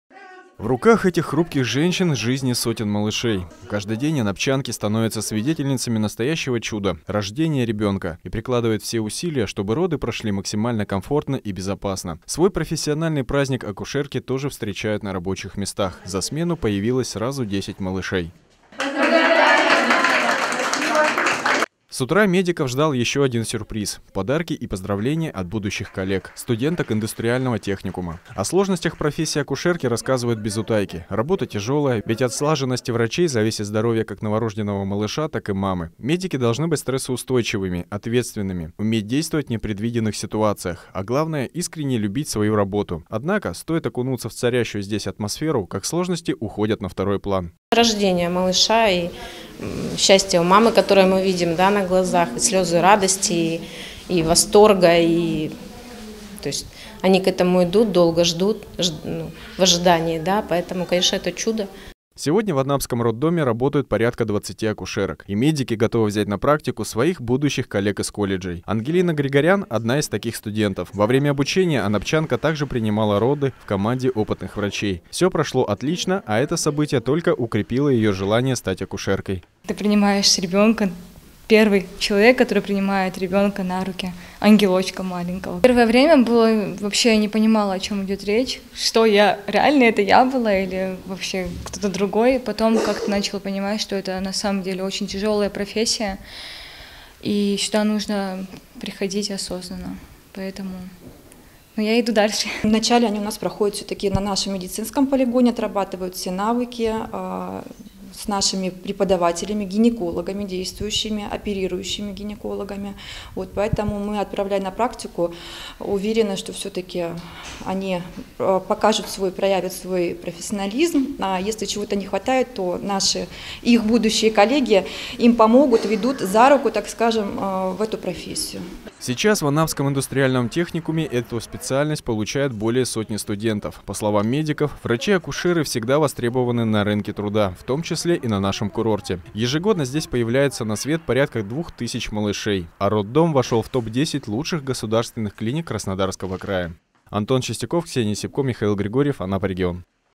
Анапские студенты поздравили врачей-акушеров с профессиональным праздником
5 мая знаковый день не только для журналистов, но и для работников роддомов. Сегодня отмечается Международный день акушерки. Впрочем, анапские врачи проводят его в рабочей атмосфере – накануне, за сутки в родильном отделении появились на свет 10 малышей. Как признались врачи, это и стало их главным подарком. А утро началось с приятного сюрприза от студентов индустриального техникума.